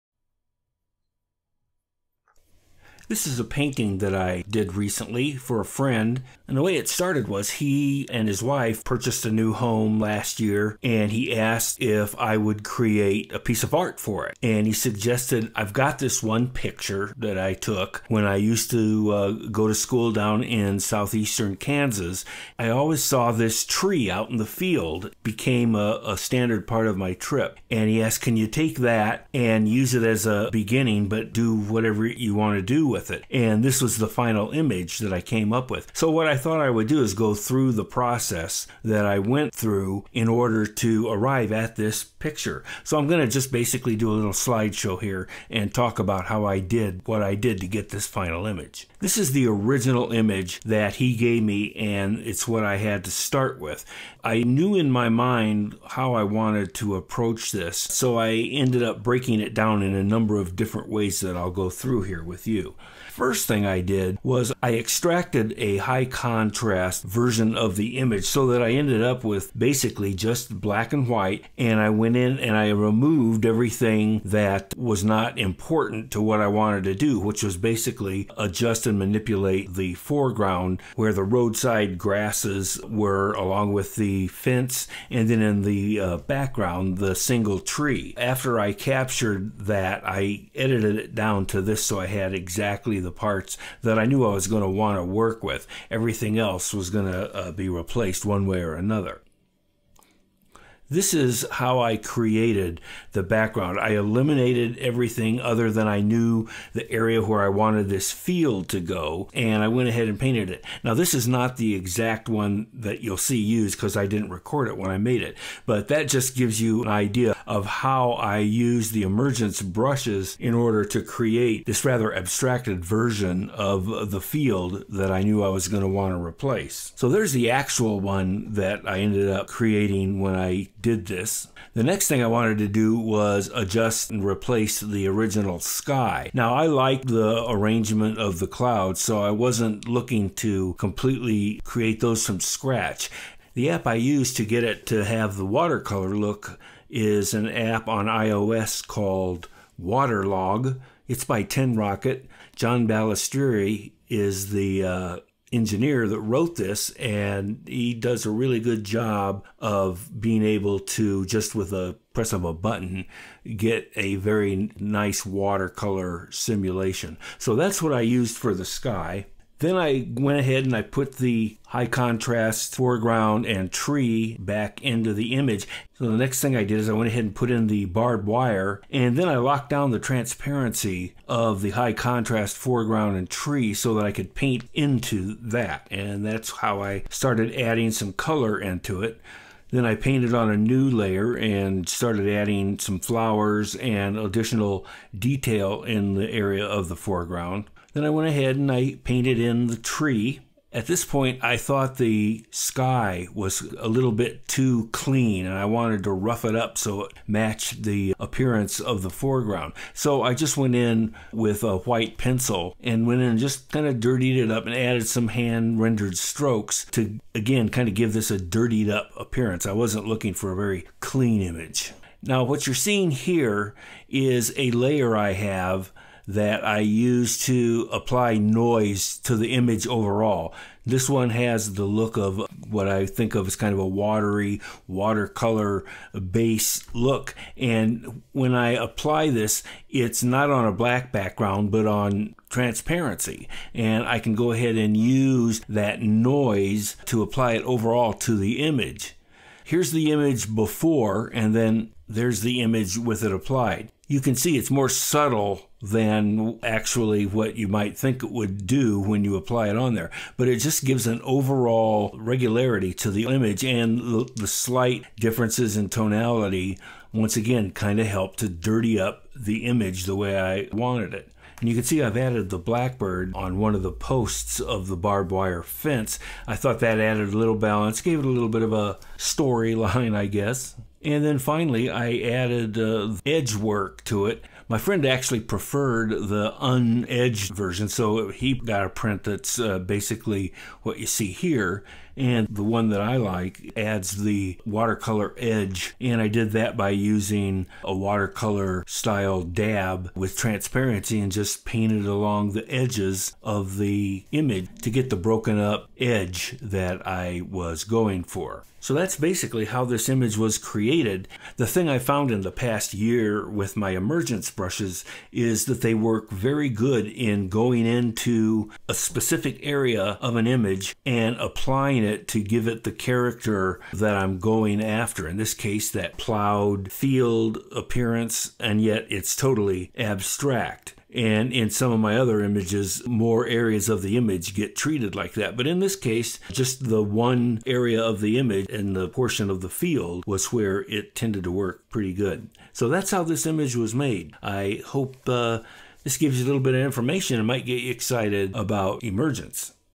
Creating “The View from Highway 69” with Emergence for Painter 2021
I recently did a painting for a friend. He and his wife recently moved into a new home and asked if I would do a painting for them. He had a photo he taken of a tree he always saw when he was going to and from college in southeastern Kansas. This video details the process I used to create the final digital painting done in Corel Painter 2021 using my Emergence brushes.